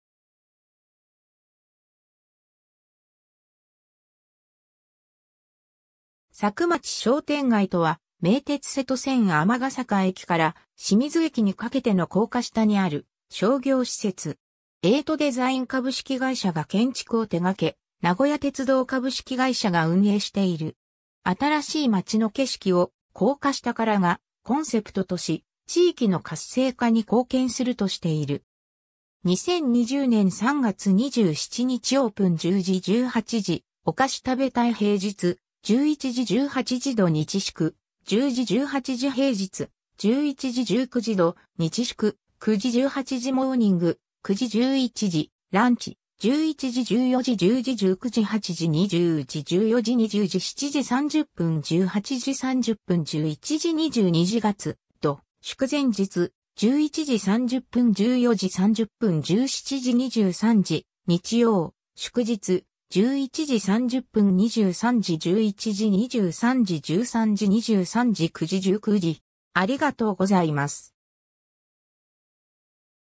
SAKUMACHI商店街
SAKUMACHI商店街（さくまちしょうてんがい）とは、名鉄瀬戸線 尼ケ坂駅から清水駅にかけての高架下にある商業施設。
 エイトデザイン株式会社が建築を手掛け、名古屋鉄道株式会社が運営している。
 「あたらしいまちの景色を、高架下から」がコンセプトとし、地域の活性化に貢献するとしている。
 2020年（令和2年）3月27日オープン 10：00～18：00（火曜日・水曜日定休） おかしたべたい - 平日：11：00〜18：00　土日祝：10：00〜18：00（第一水曜日定休） 平日：11：00～19：00　土日祝：9：00～18：00（入店受付は閉店60分前まで、水曜日定休） モーニング（土日祝）：9：00～11：00、ランチ：11：00～14：00（入店受付は閉店60分前まで） 10：00～19：00（夏期は20：00まで） 8：00～20：00（モーニング：8：00～11：00、ラストオーダー：19:00） 14：00～20：00 7：30～18：30（日曜休業） 11：00～22：00 月～土・祝前日：11：30～14：30 （ラストオーダー・料理：14：00、ドリンク：14：00） 17：00～23：00 （ラストオーダー・料理：22：30、ドリンク：22：30） 日曜・祝日：11：30～23：00 （ラストオーダー・料理：22：30、ドリンク：22：30） （第1・3・5月曜日、第2・4火曜日定休） 11：00～23：00（月曜日・月曜祝日なら火曜日定休） 13：00～23：00（火曜不定休） 9：00～19：00（月・火曜日定休）